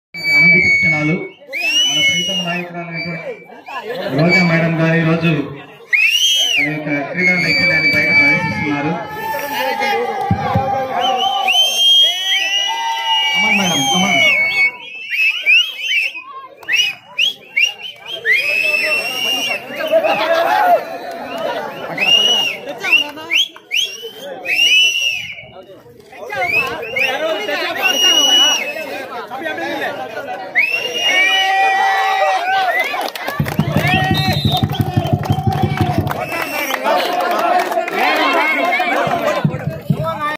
కబడ్డీ ఆడిన రోజా అక్క roja akka kabbadi video/ minister Rk roja play with students/ vivo pro kabbadi
MLA Roja Kabaddi playing with boys, actress Roja Kabaddi playing video
 thanks watching for this video
   By
Siri pro Channel